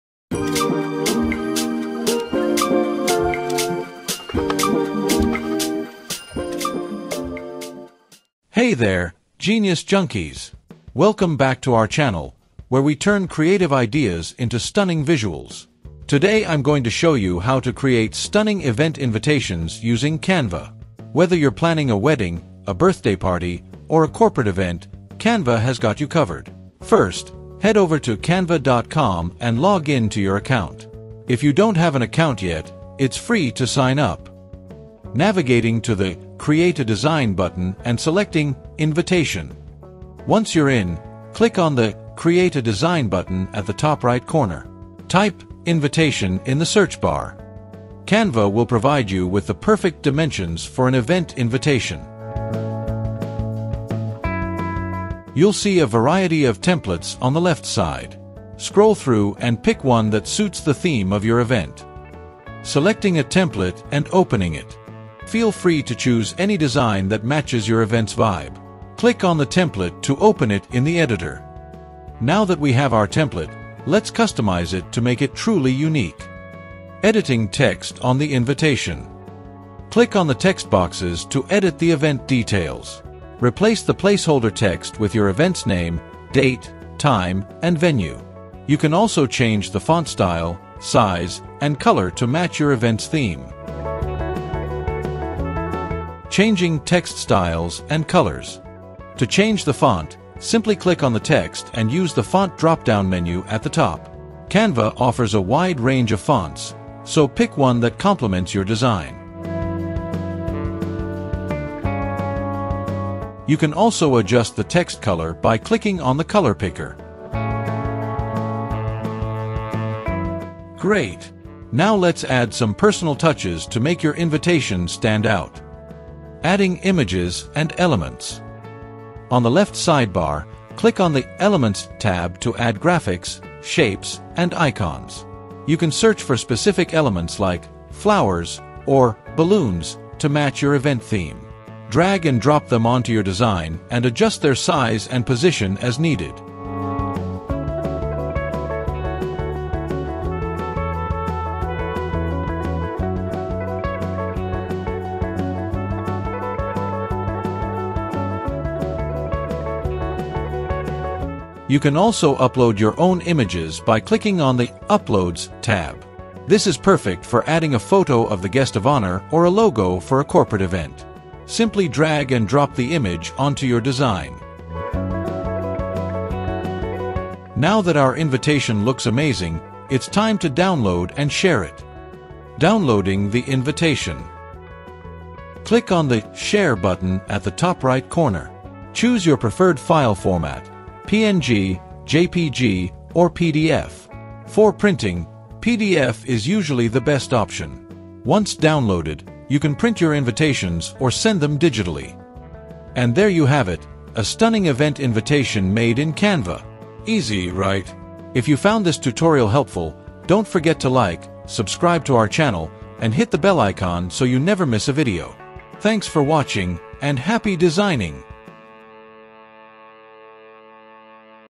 How to Create Stunning Event Invitations with Canva | Step by Step Guide
🎉 Learn how to design stunning event invitations with Canva in this step-by-step guide! Whether you're planning a birthday, wedding, corporate event, or party, Canva's customizable templates and tools make it easy to create professional and eye-catching invitations. 🎨💌

In this tutorial, you’ll discover: ✅ How to choose and customize event invitation templates in Canva
🎨 Adding personal touches like images, logos, and text
📏 Design tips for creating invitations that stand out
📥 Exporting your invitations for print or digital sharing

No matter the occasion, Canva helps you create beautiful event invitations that leave a lasting impression. Perfect for anyone looking to make their invites unique and stylish! 🌟